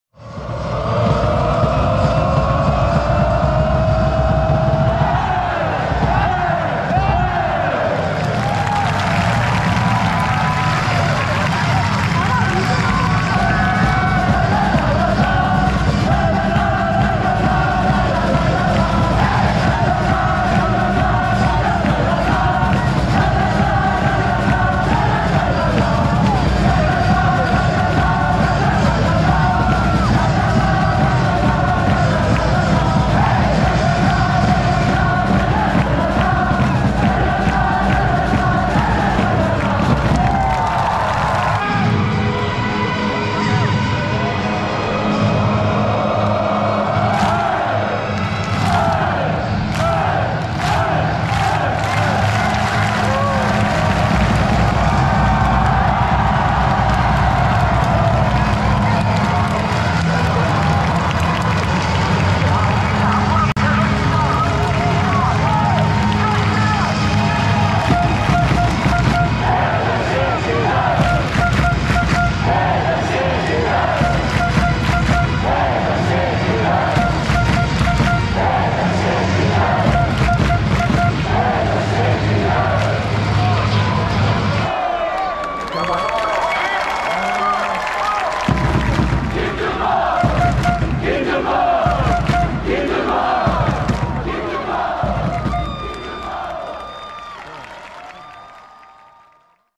[파이널B-1] 퍼플벨, 김준범 대전박수 | 대전하나시티즌 | vs전북 | 20241019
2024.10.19. 토. 2PM. 전주월드컵경기장

K리그1 파이널B 첫 번째 경기.
대전하나시티즌 2-0 전북현대

인스타 ekj.tv

퍼가실 때는 출처와 링크 표기 바랍니다.